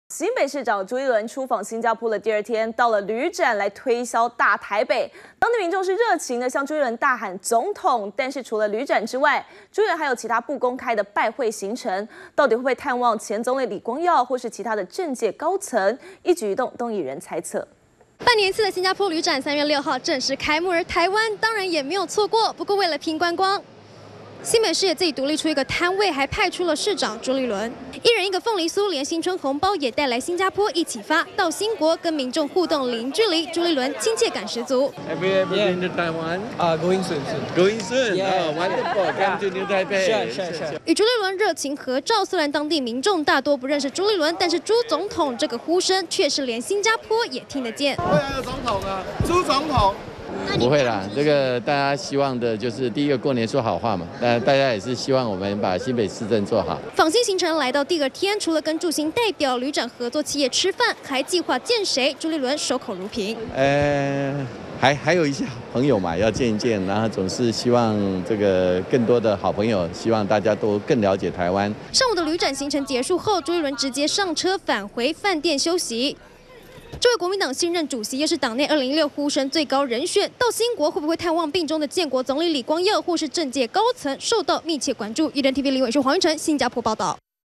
【2015.03.07】訪星第二天 朱立倫推銷大台北 -udn tv
新北市長朱立倫，出訪新加坡的第二天，到旅展，推銷大台北。當地民眾，也熱情地向朱立倫大喊總統。但除了旅展之外，朱立倫還有其他不公開的拜會行程，會不會探望星國前總理李光耀，或是其他政界高層，一舉一動，動見觀瞻。完整內容 , FB , Twitter , G+